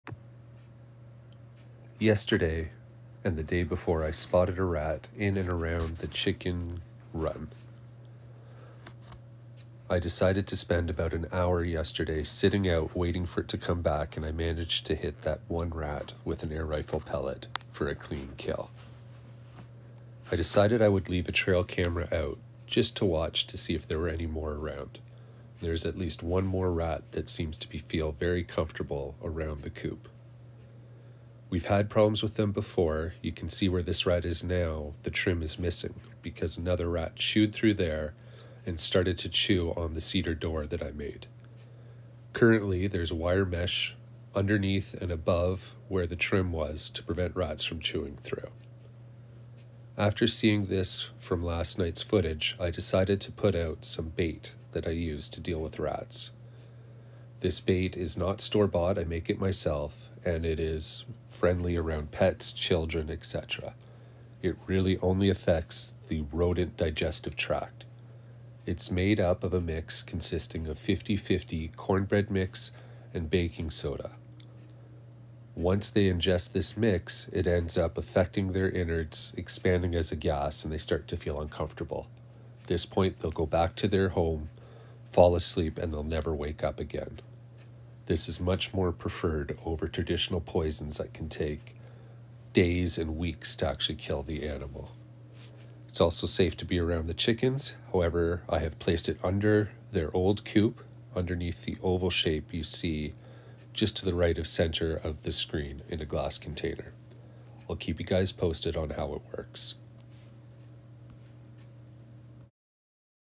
Rat-a-ewwie. Rat issues around chicken coop/run and safe way to deal with them
￼￼￼￼I am a big fan of the most ethical pest control possible.  Relocation is rarely a good idea as just makes the pest someone else’s problem who might poison them or worse.  I’m this video I discuss some of the issues and the best way I’ve found to deal with them without traps or poison that can effect non target animals. A dry mix of 50/50 cornbread mix and baking soda acts like a vinegar volcanoe and only affects rodent digestive tracks.   Will not harm cats or dogs or other non rodent pests though I do my best to make sure birds won’t ingest it too.  If it does kill a rat and say an eagle or a hawk ears that rat it will not effect that predator. Good luck with your rodent problem!  This has worked in my workshops, sheds and around the doors of my house with the rat population no longer seen and no messy poisons or traps used. :-)